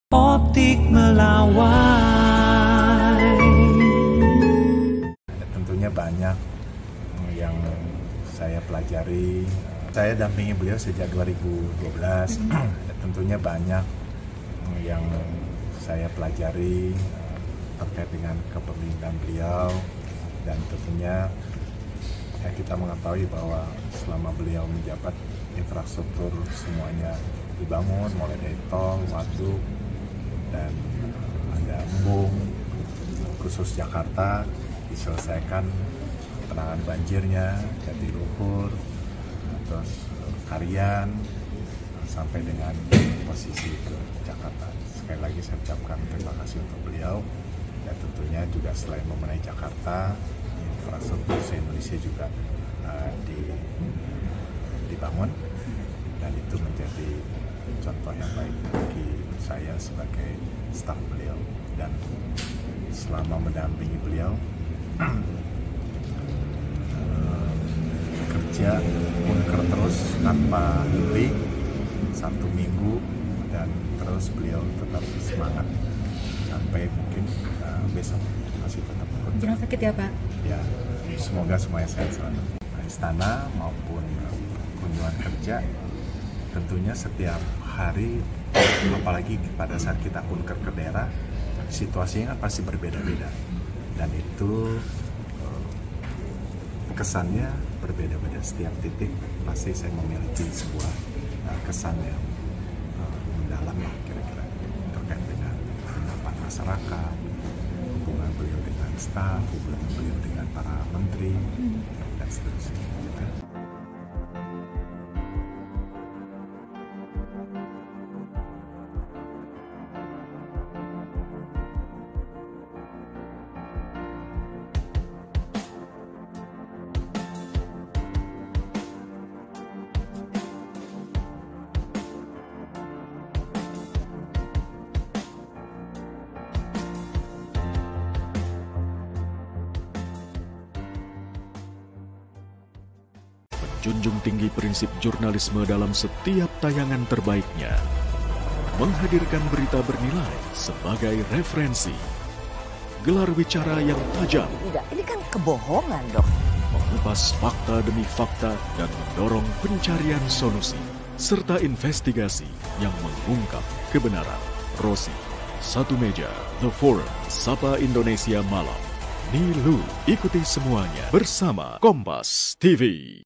Heru Budi Dampingi Jokowi 12 Tahun Belajar Banyak dari Kepemimpinan Beliau
JAKARTA, KOMPAS.TV - Kepala Sekretariat Presiden (Kasetpres) Heru Budi Hartono mengungkapkan kesan mendalam setelah mendampingi Presiden Joko Widodo (Jokowi) selama 12 tahun, dimulai dari masa jabatan di Pemerintah Provinsi DKI Jakarta pada 2012.

Heru Budi menyatakan, banyak pelajaran berharga yang diperolehnya terkait model kepemimpinan Jokowi.

Selain itu, Heru Budi mengenang Jokowi sebagai sosok yang memiliki segudang aktivitas padat khususnya terkait kunjungan kerja ke berbagai lokasi.